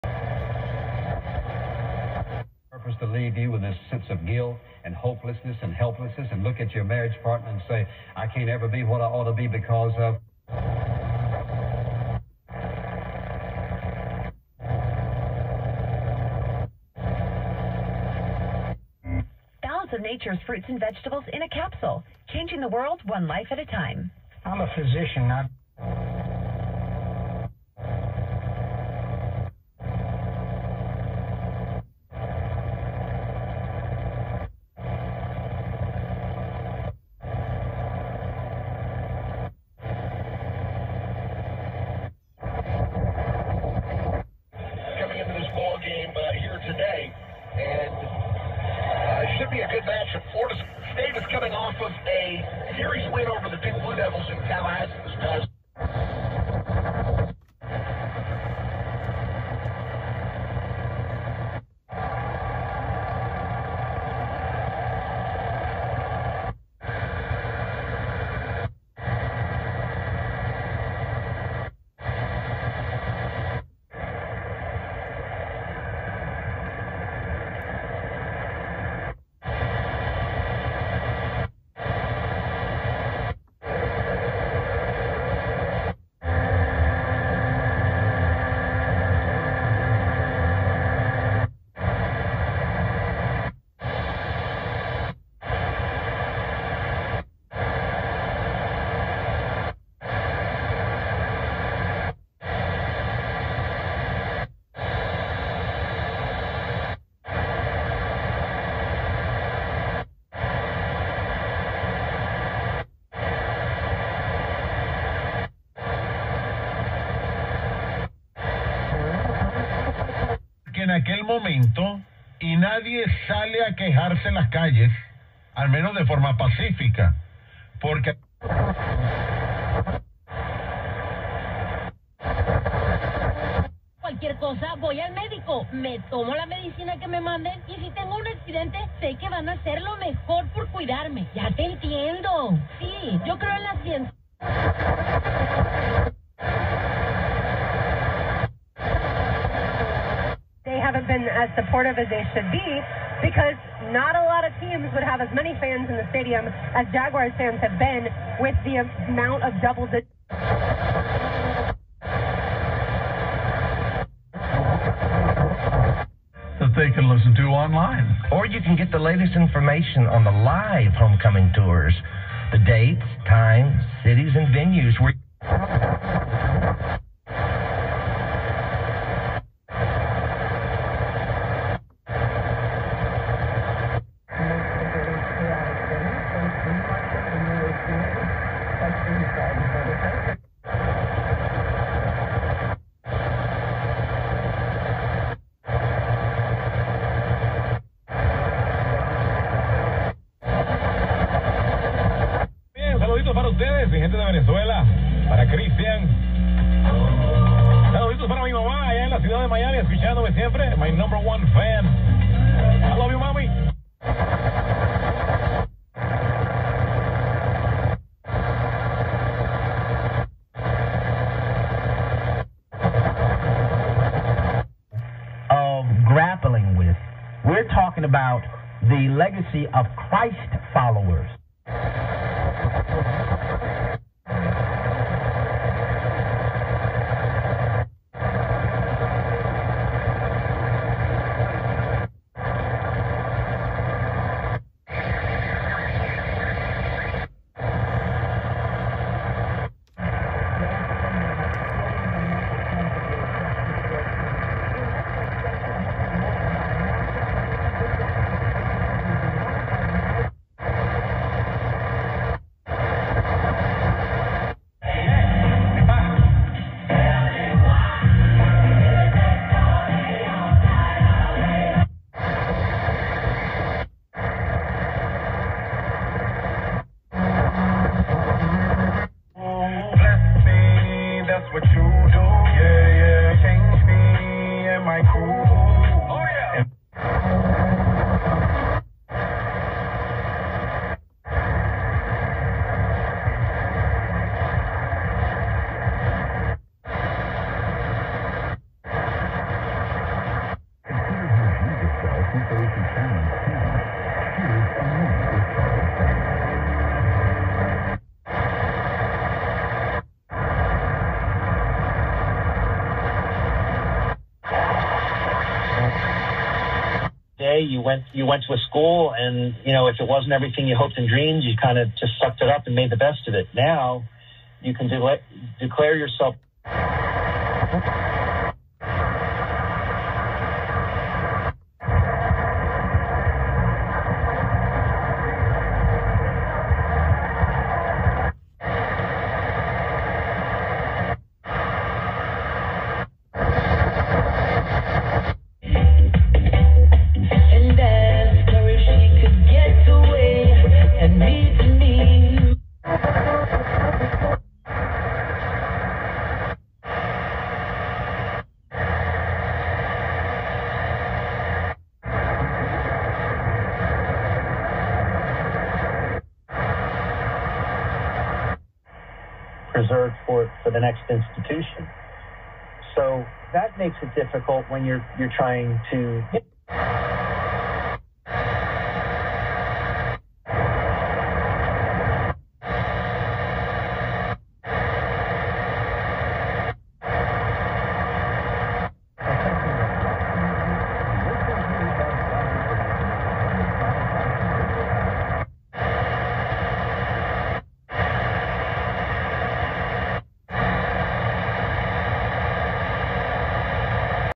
Jacksonville, Florida Daytime AM Bandscan #1 - March 29, 2022
Still enough daylight for a local scan. (I have heard the ground conductivity in the Southeast is not the greatest; even an AM station from 30 miles away may not have great reception the next county over.)

550-WAYR/Fleming Island
600-WBOB/Jacksonville
690-WOKV/Jacksonville
800-WPLK/Palatka
930-WFXJ/Jacksonville
970-WNNR/Jacksonville
1010-WJXL/Jacksonville Beach
1050-WROS/Jacksonville
1100-WCGA/Woodbine, GA
1160-WEWC/Callahan
1220-WKTZ/Jacksonville
1290-(Very faintly) WTKS/Savannah
1320-WJNJ/Jacksonville
1360-WCGL/Jacksonville
1400-Supposed to be WZAZ/Jacksonville
1420-WAOC/St. Augustine
1460-WQOP/Jacksonville
1490-A very faint WBGA/Brunswick, GA
1530-WYMM/Jacksonville
1600-WZNZ/Atlantic Beach
1670-Likely WMGE/Macon, GA